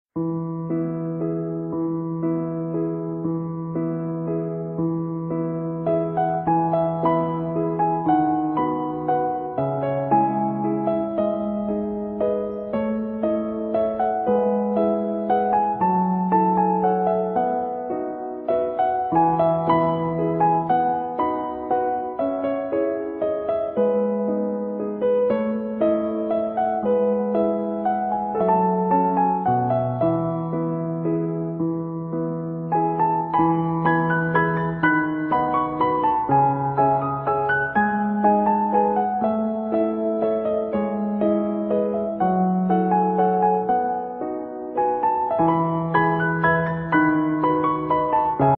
Painting a scene from Studio Ghibli's Spirited Away - No-face on the Bridge | Teaser
In this short video I share my simple painting of a memorable scene from Studio Ghibli's Spirited Away, where Chihiro encounters No-face for the first time on a bridge in the spirit world. It was my second Ghibli painting ever and I did my best to bring to life the hauntingly beautiful atmosphere of the scene.